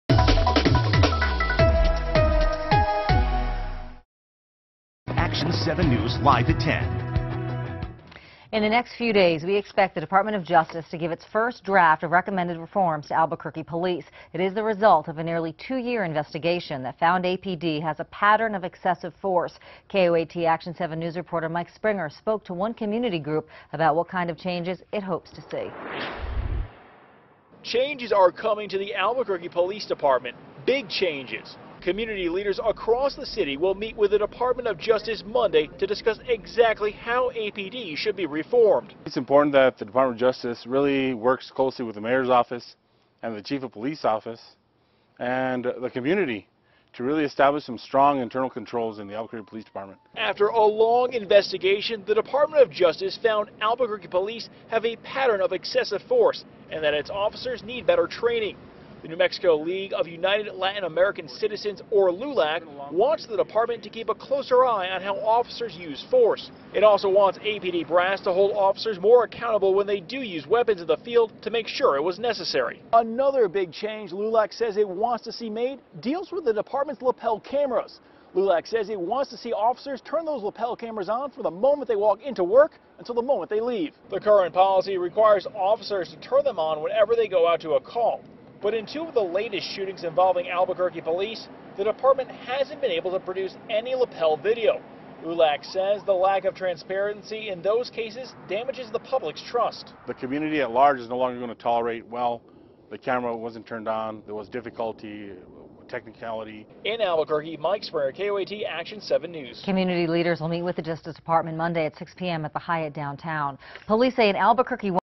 DOJ to release recommendations for APD reform this week
Albuquerque community leaders are expected to meet with the U.S. Department of Justice just before the department releases its recommendation for reforms at the Albuquerque Police Department this week.